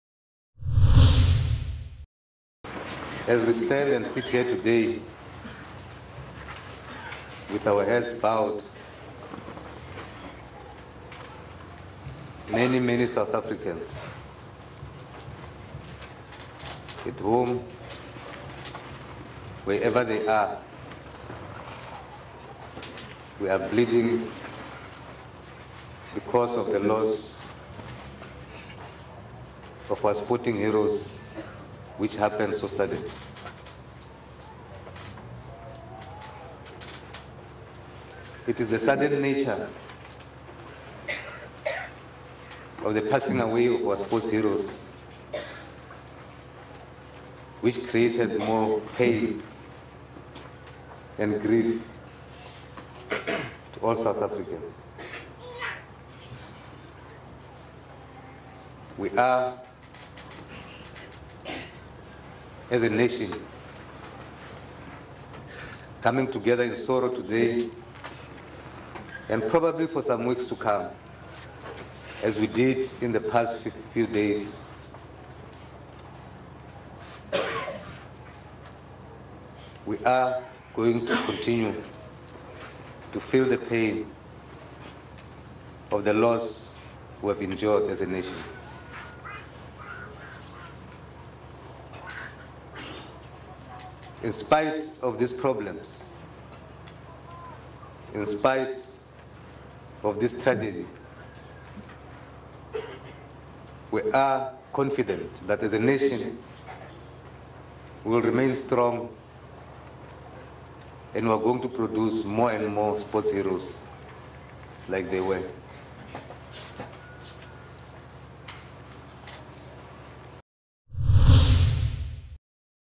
Chabane speaks of SA's grief at Mulaudzi funeral
Limpopo, 1 November 2014 - Minister of Service and Administration Collins Chabane says once the grief has passed, South Africa will look forward to creating stars of the future. Chabane was speaking at the funeral of Olympic medallist Mbulaeni Mulaudzi.